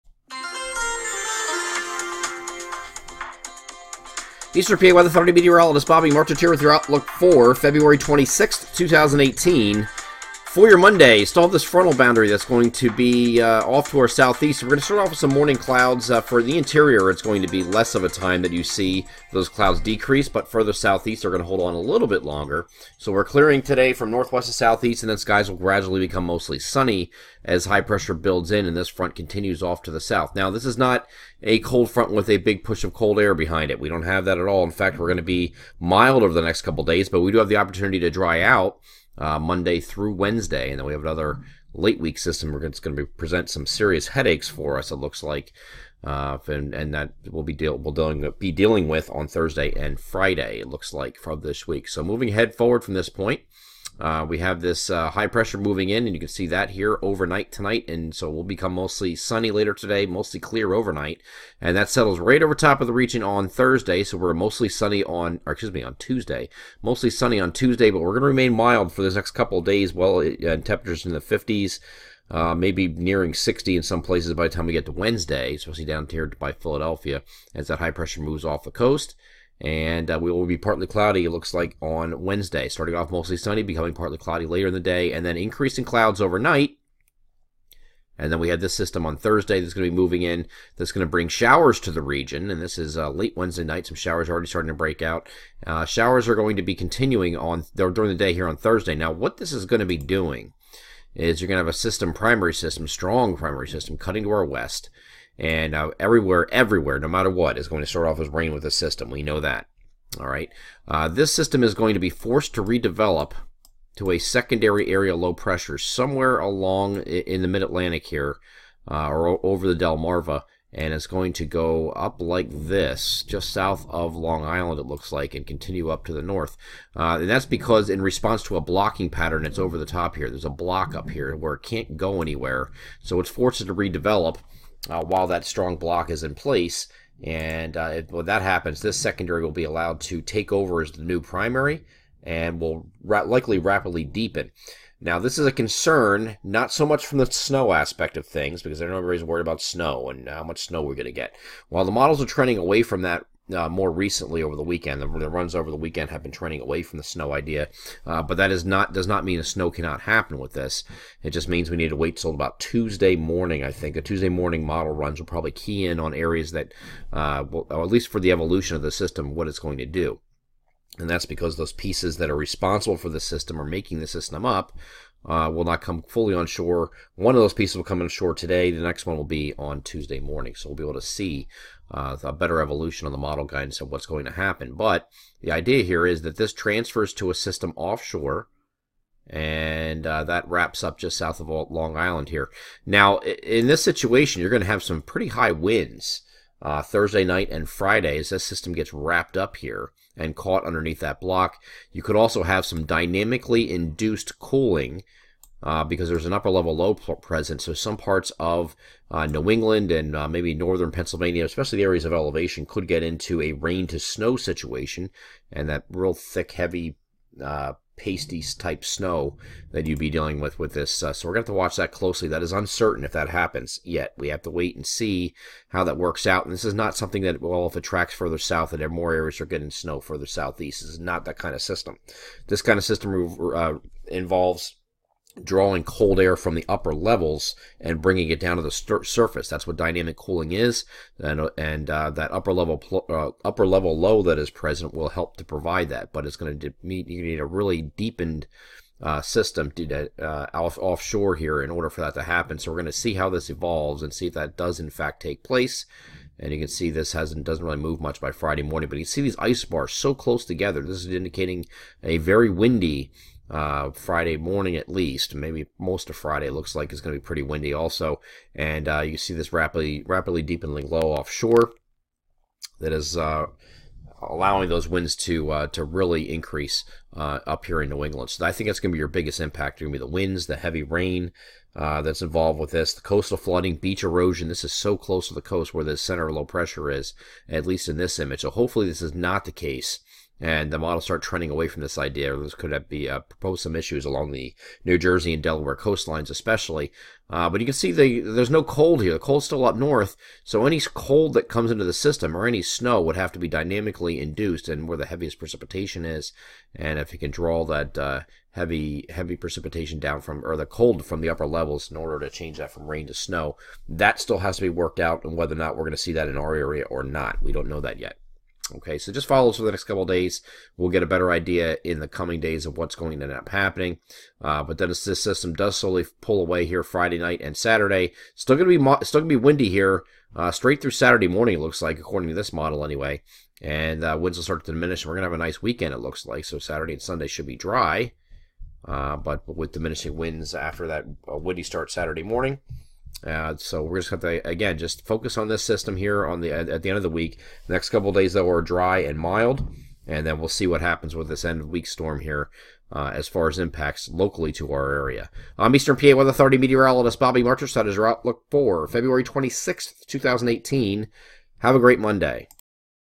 Daily forecast video for Monday February 26th, 2018
EPAWA's video forecast for Monday 2/26 and the week ahead